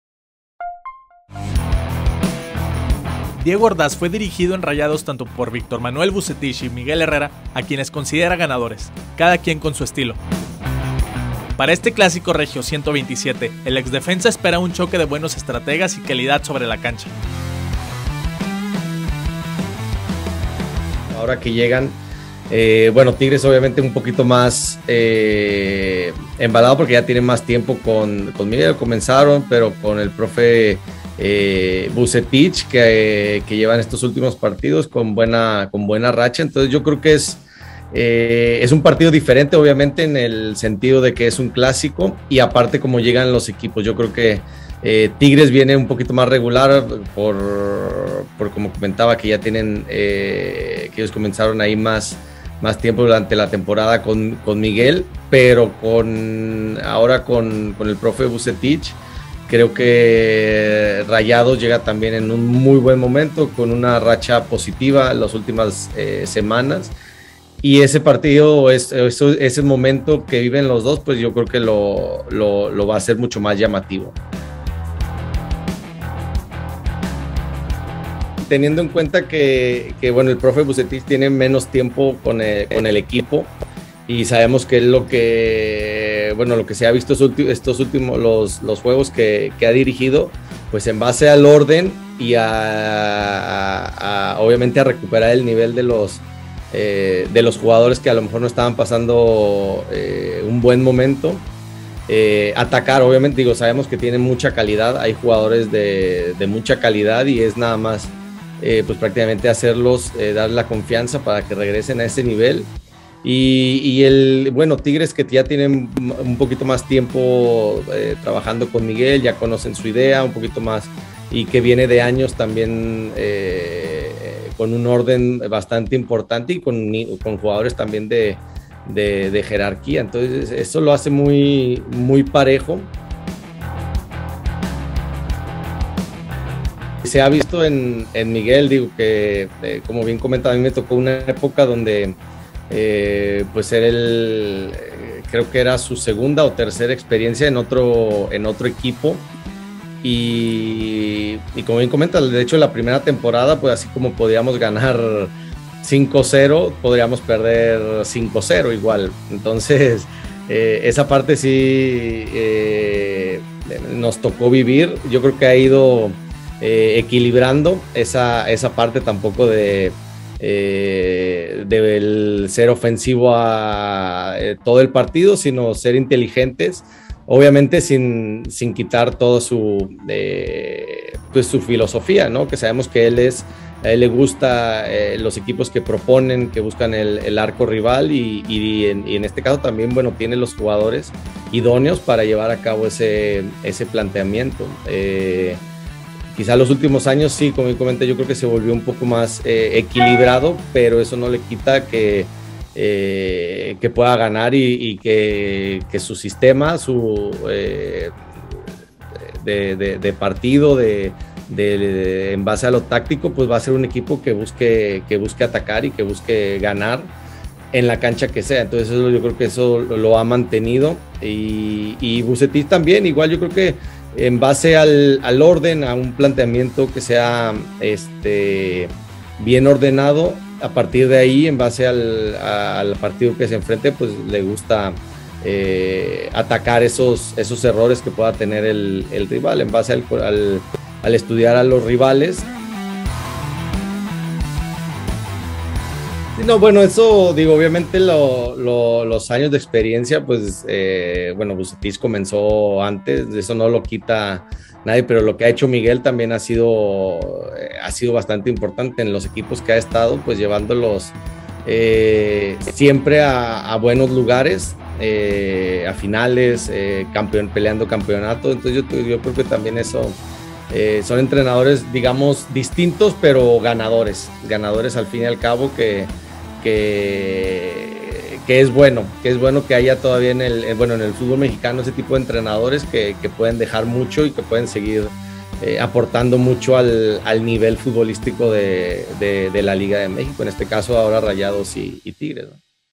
Clásico Regio: Son Vucetich y Herrera técnicos ganadores, según Diego Ordaz
Diego Ordaz fue dirigido por Vucetich y por el "Piojo"; así se refiere a cada uno de ellos.